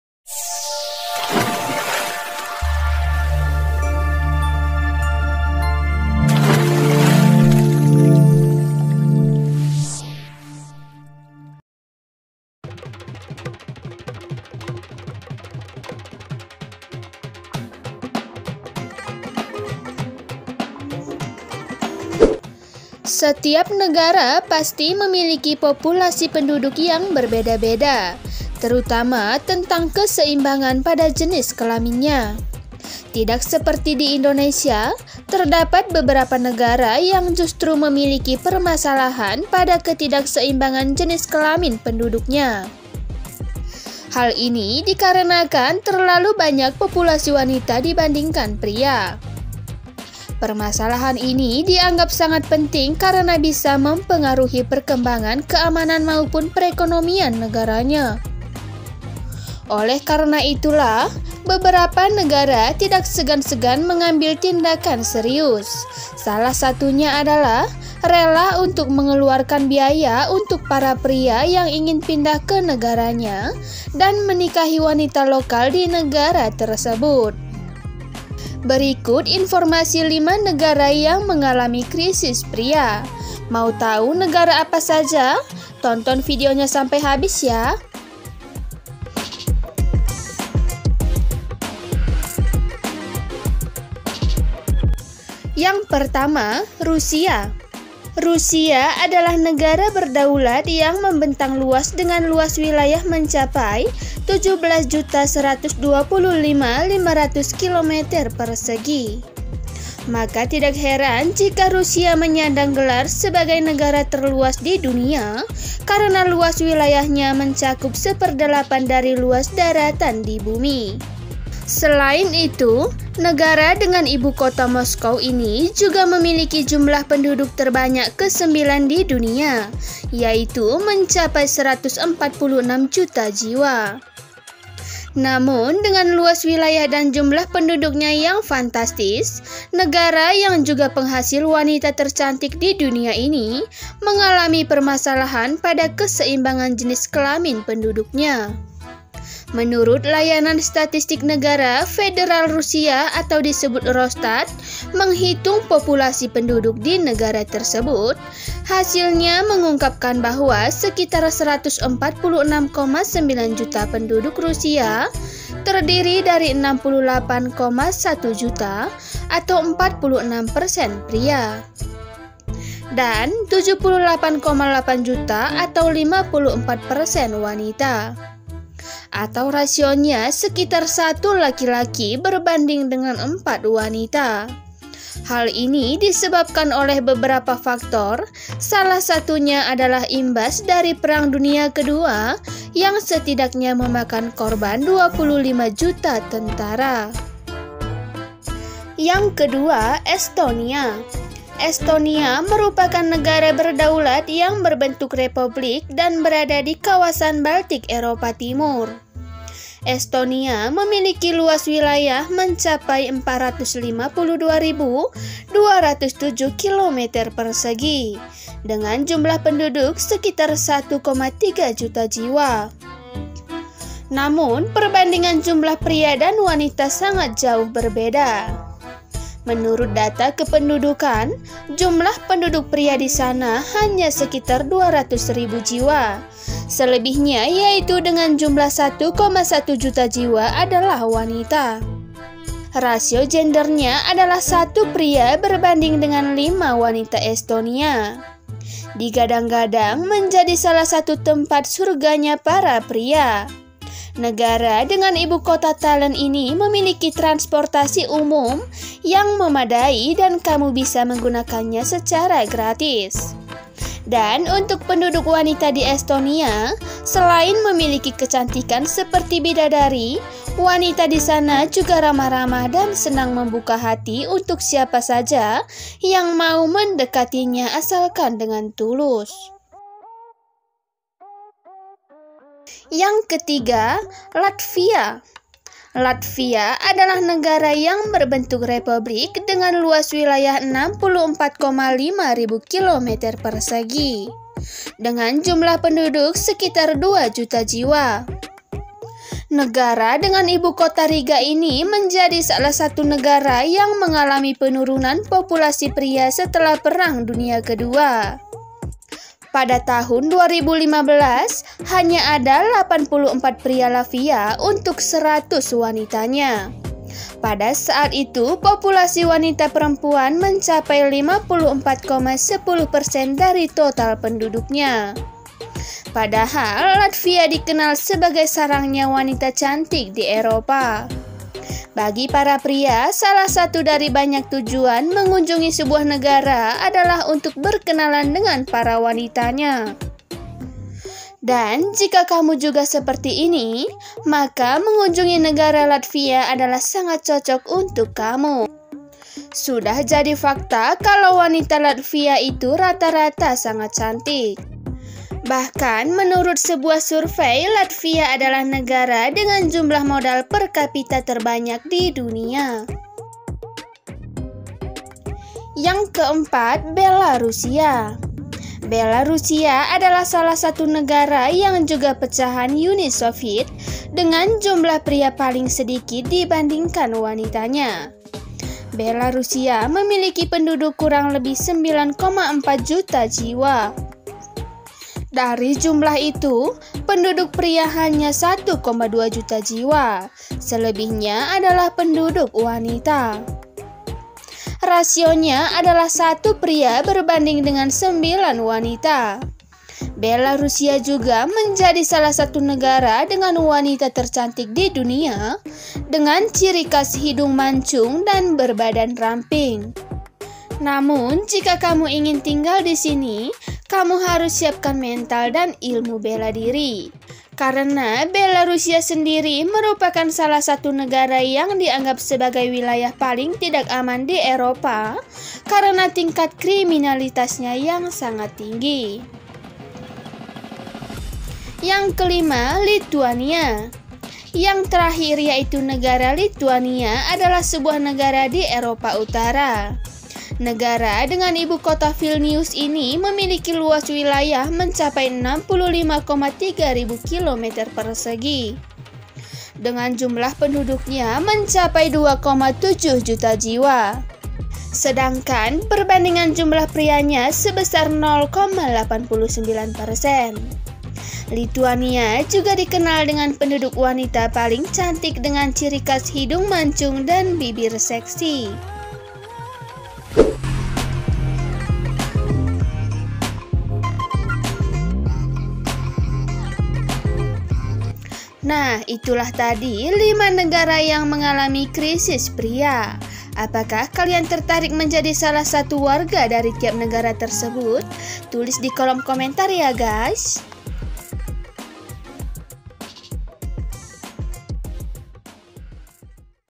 Dihuni Oleh Wanita Cantik Tapi Krisis Pria! Inilah 5 Negara Paling Krisis Pria Di Dunia
Setiap negara pasti memiliki populasi penduduk yang berbeda-beda. Terutama tentang keseimbangan pada jenis kelaminya.
Tidak seperti di Indonesia, terdapat beberapa negara yang justru memiliki permasalahan  pada ketidakseimbangan jenis kelamin penduduknya.
Hal ini dikarenakan terlalu banyak populasi wanita dibandingkan pria. Permasalahan ini dianggap sangat penting karena bisa mempengaruhi perkembangan keamanan maupun perekonomian negaranya.
Oleh karena itulah, beberapa negara tidak segan-segan mengambil tindakan serius. Salah satunya adalah rela untuk mengeluarkan biaya untuk para pria yang ingin pindah ke negaranya dan menikahi wanita lokal di negara tersebut.
Penasaran negara apa saja?